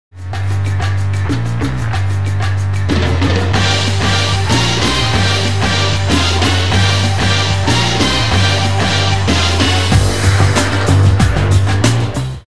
Wielka Gala Otwarcia - Zwiastun 1
Takie mignięcie sekund z Wspaniałego Spektaklu jaki odbędzie się 3 listopada 2008 roku w NCK w Krakowie, jako przedsmak tego co będzie można zobaczyć

F0036